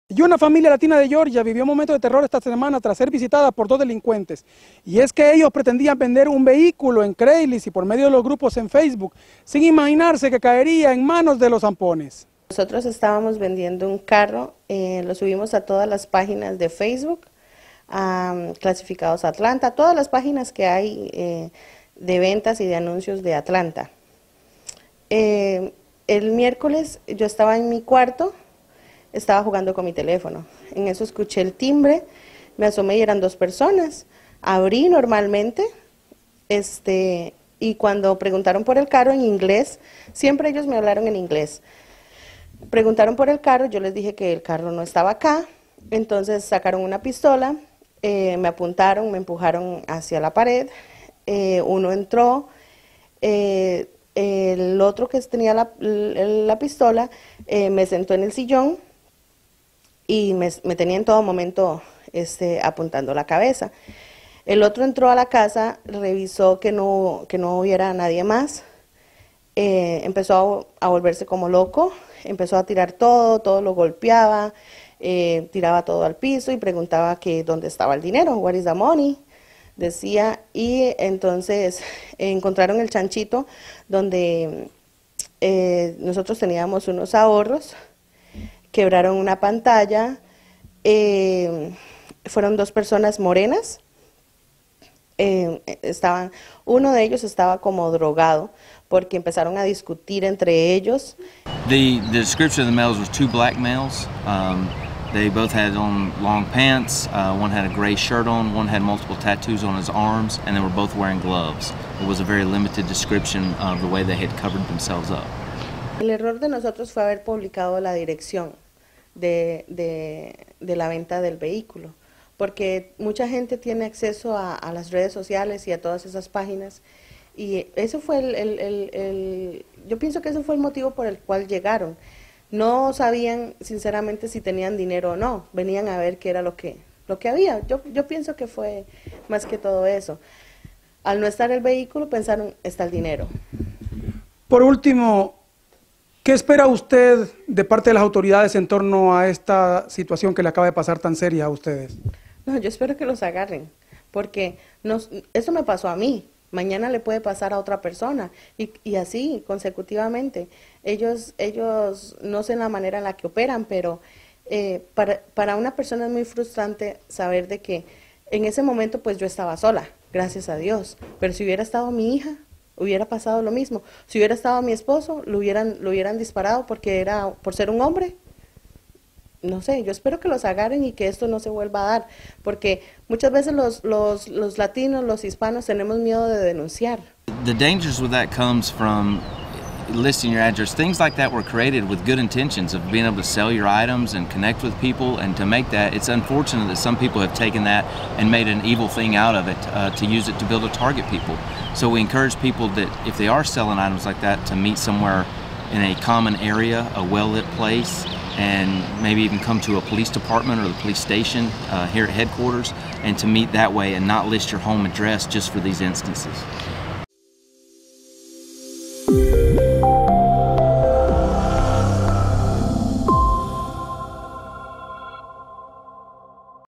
Familia hispana asaltada
La mujer, de origen costarricense y cuya identidad pidió que no se revelara por temor a represalias, contó en exclusiva que los sujetos simularon estar interesados en comprar su auto.